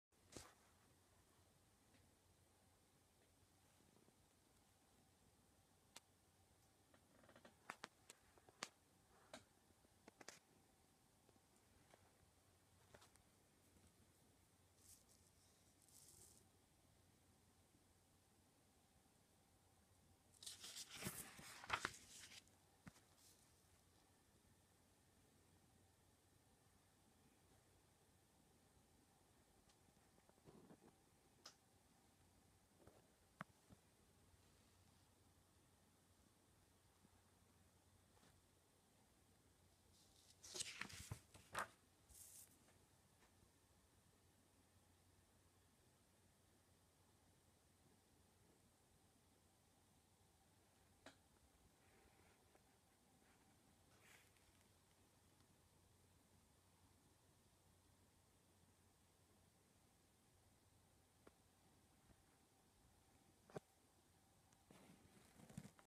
Vintage ephemera 1907 article christmas at sandringham
Another great piece of royalty ephemera from bonsuivit and themoonlightergroup